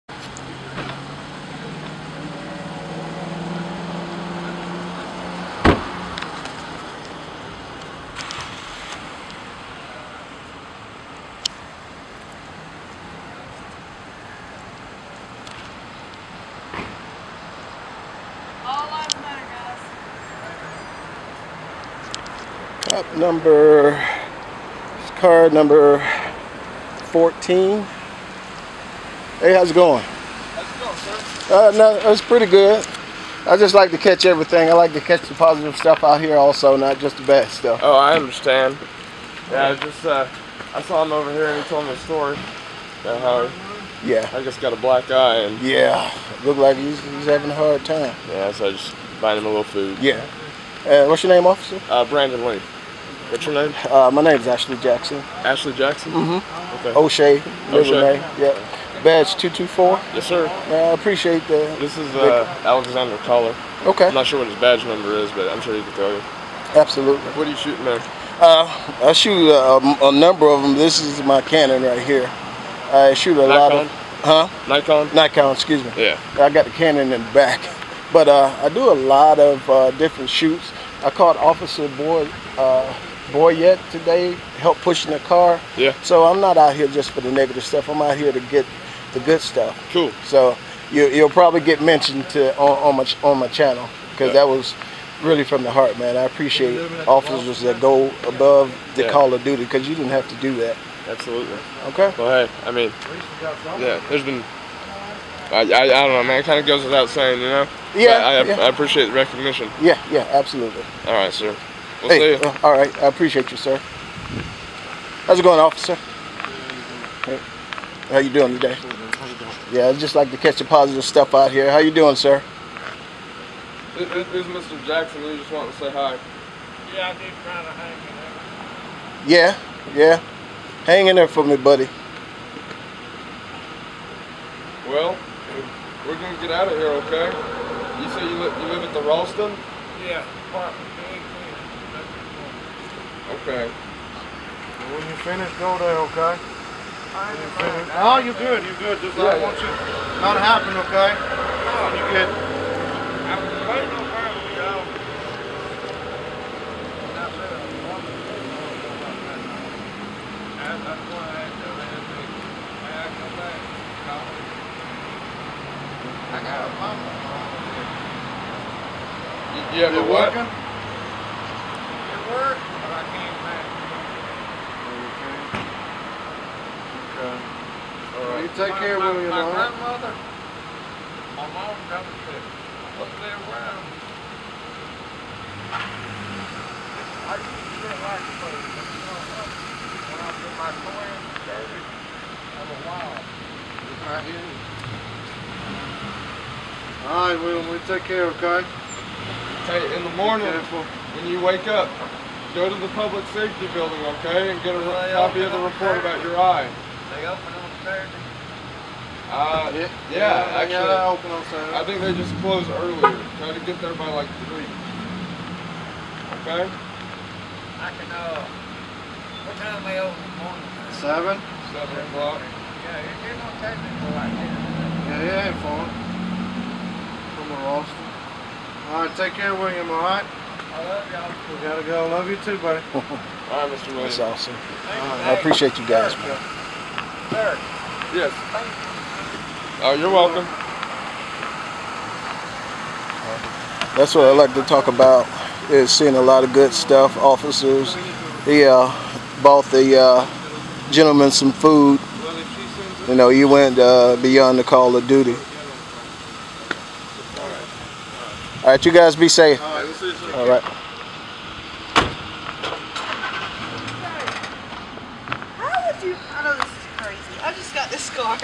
Columbus, Officer Lee Badge #224 Protecting and serving
Columbus Ga Officer Lee Badge #224 going well beyond his call of duty, feeding a man seriously down on his luck, Officer Lee is truely serving and protecting our community